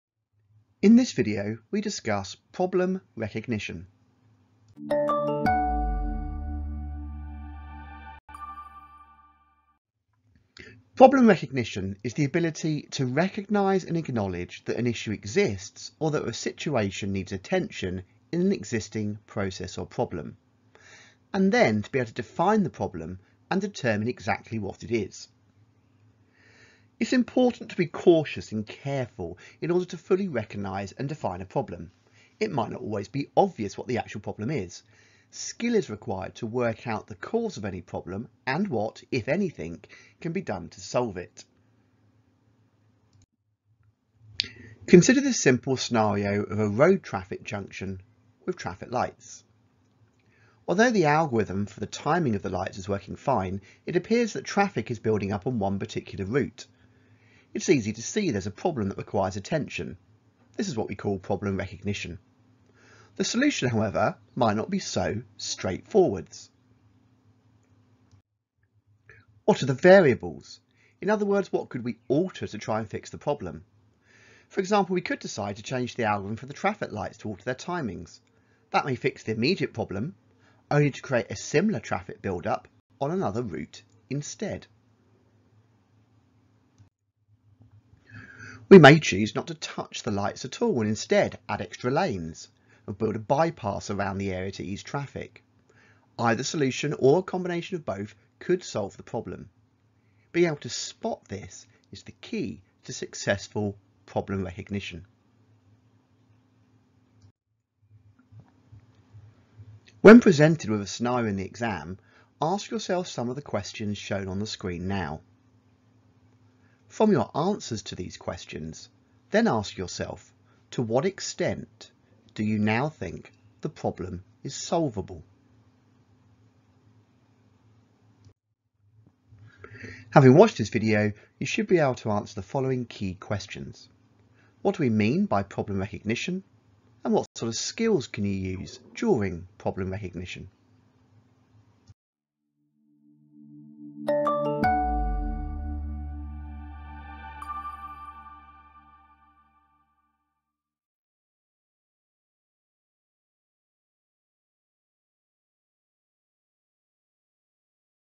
140. OCR A Level (H446) SLR24 - 2.2 Problem recognition
A Level 2.2.2c

00:00 Intro
00:10 Problem Recognition: What is Problem Recognition?
00:43 A Scenario
01:48 Problem Recognition Questions
02:08 Key Questions
02:22 Outro